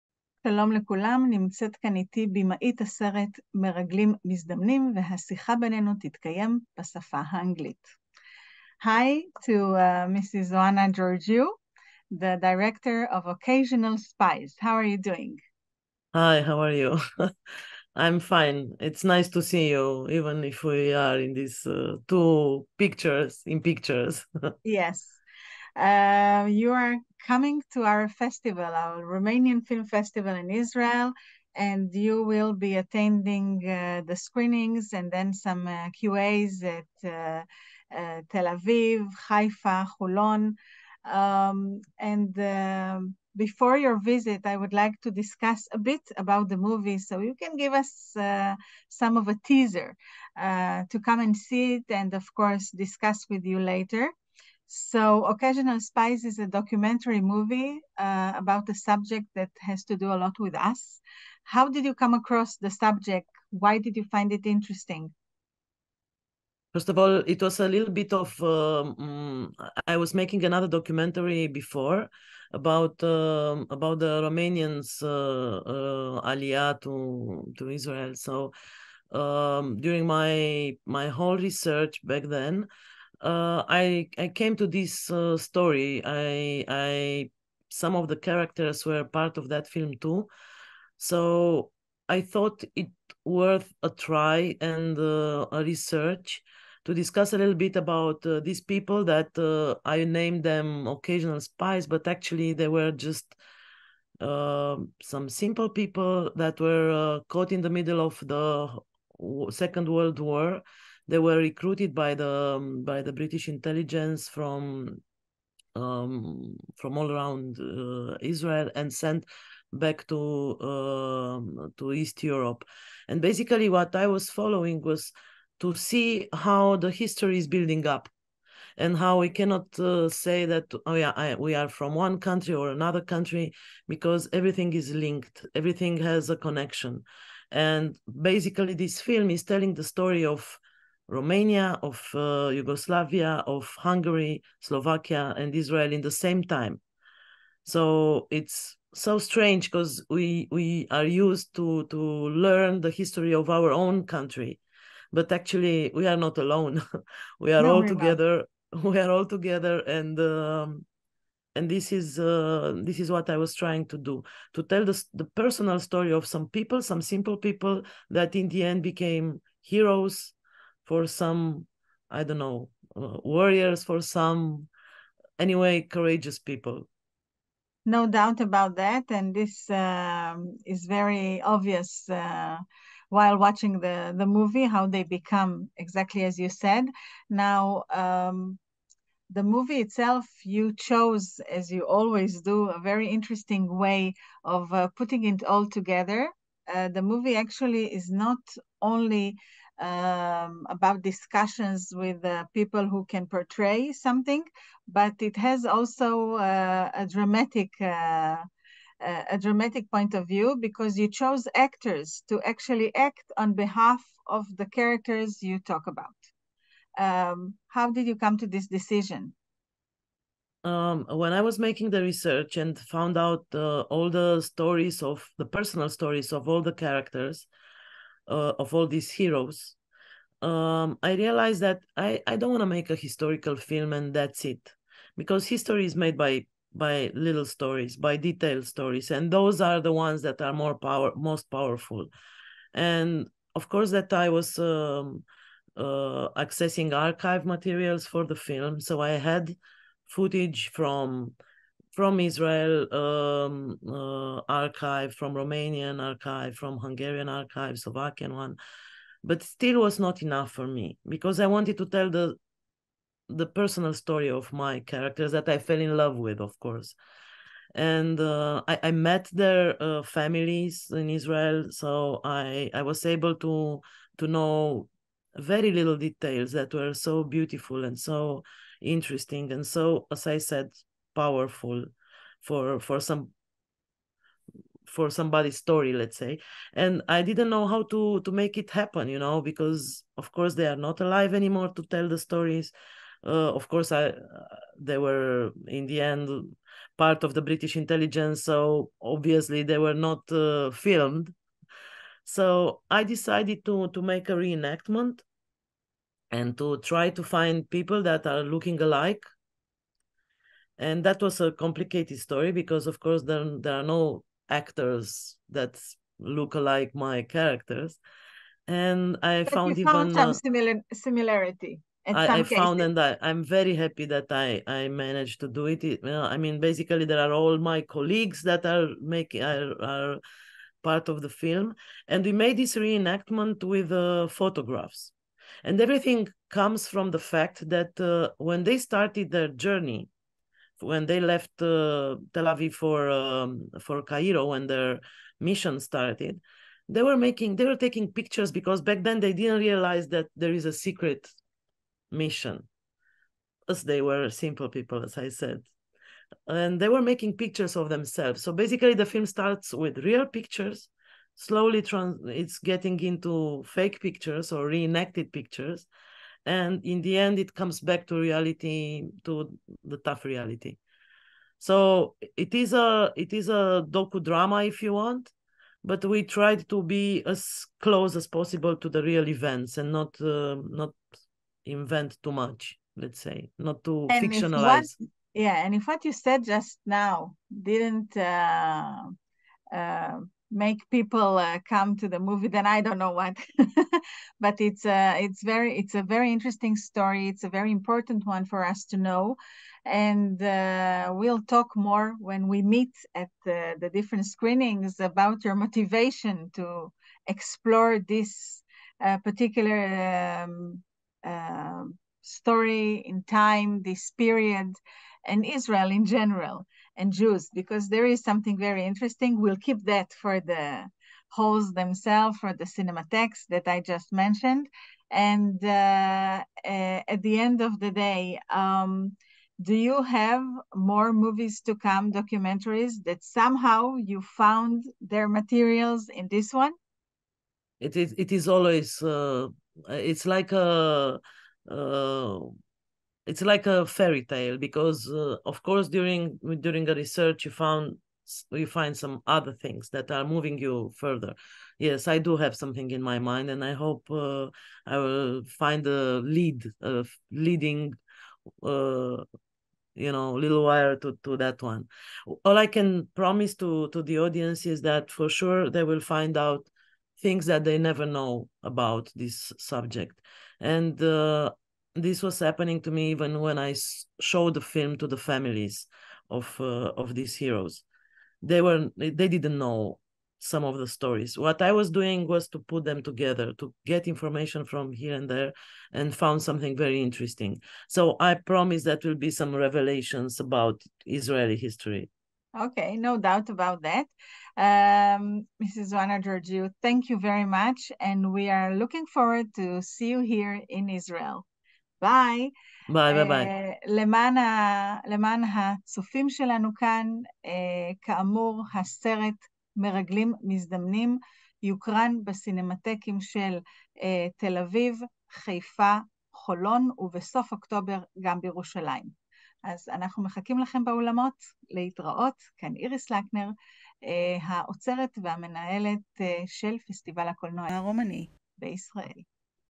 𝐎𝐚𝐧𝐚 𝐆𝐢𝐮𝐫𝐠𝐢𝐮 𝐢̂𝐧 𝐝𝐢𝐚𝐥𝐨𝐠 𝐜𝐮 𝐈𝐫𝐢𝐬 𝐋𝐚𝐜𝐤𝐧𝐞𝐫
Regizoarea 𝐎𝐚𝐧𝐚 𝐆𝐢𝐮𝐫𝐠𝐢𝐮 în dialog cu Iris Lackner despre documentarul 𝑆𝑝𝑖𝑜𝑛𝑖 𝑑𝑒 𝑜𝑐𝑎𝑧𝑖𝑒, prezentat la Cinematecile din Tel Aviv (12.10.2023) , Ierusalim (31.10), Haifa (15.10) și Holon (11.10), cu ocazia  𝐅𝐞𝐬𝐭𝐢𝐯𝐚𝐥𝐮𝐥𝐮𝐢 𝐅𝐢𝐥𝐦𝐮𝐥𝐮𝐢 𝐑𝐨𝐦𝐚̂𝐧𝐞𝐬𝐜 𝐢̂𝐧 𝐈𝐬𝐫𝐚𝐞𝐥, organizat de ICR Tel Aviv în colaborare cu MovieMania by Iris Lackner.
Director 𝐎𝐚𝐧𝐚 𝐆𝐢𝐮𝐫𝐠𝐢𝐮 in dialogue with Iris Lackner about the documentary 𝑂𝑐𝑐𝑎𝑠𝑖𝑜𝑛𝑎𝑙 𝑆𝑝𝑖𝑒𝑠 shown in the Cinematheques of Tel Aviv (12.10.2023), Jerusalem (31.10), Haifa (15.10) and Holon (11.10) within the 𝐑𝐨𝐦𝐚𝐧𝐢𝐚𝐧 𝐅𝐢𝐥𝐦 𝐅𝐞𝐬𝐭𝐢𝐯𝐚𝐥 𝐢𝐧 𝐈𝐬𝐫𝐚𝐞𝐥, organized by the Romanian Cultural Institute in Tel Aviv, in collaboration with MovieMania by Iris Lackner.